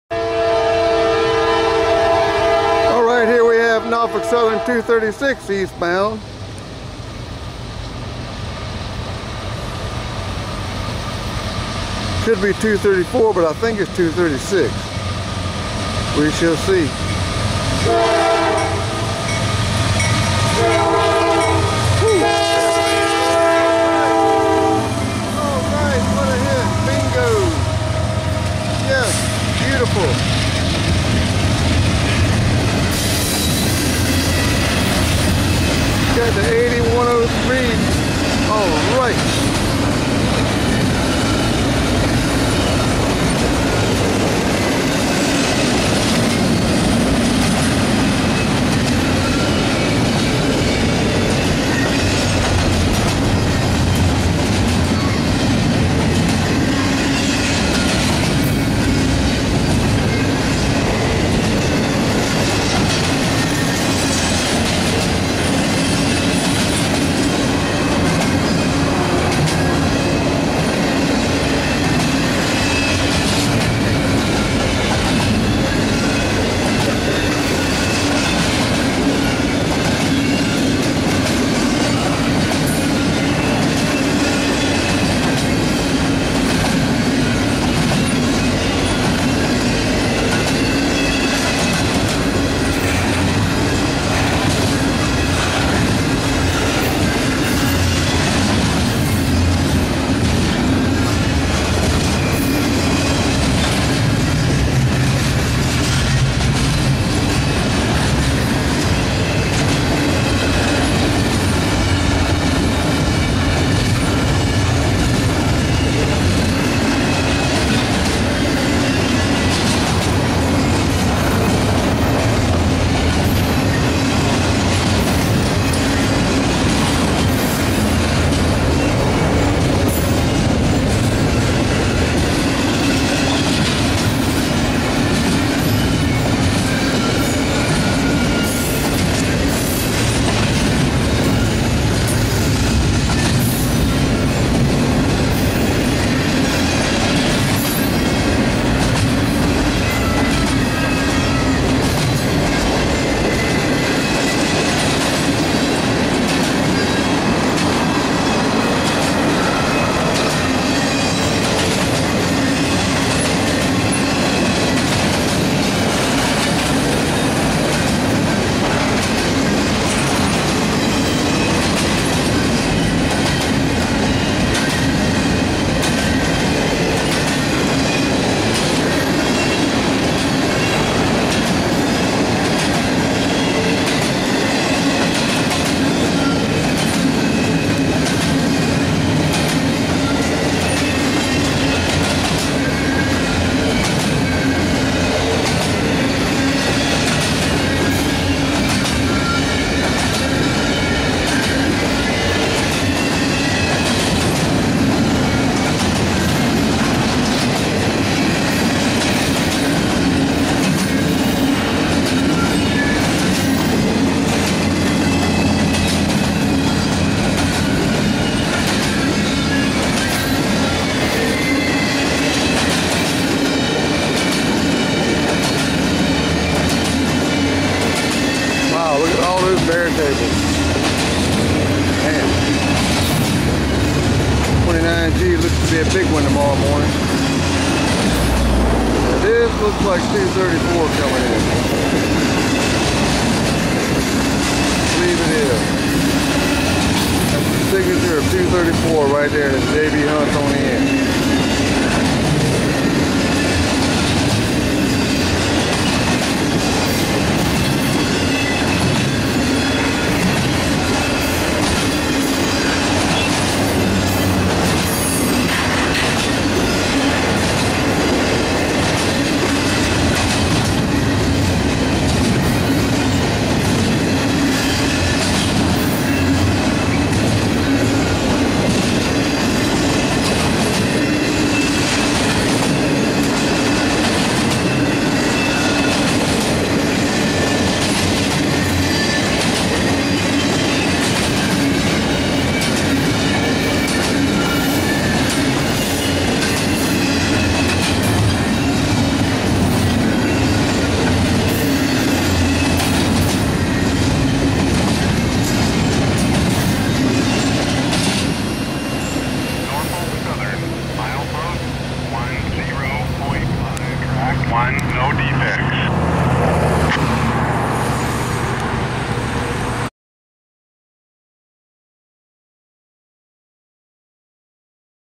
NS 234 - 12
NS 4231 (GE AC44C6M) and NS 8103 (GE ES44AC N&W Heritage Unit) leads NS 234 east on 08/13/2019 at 16:47 pm.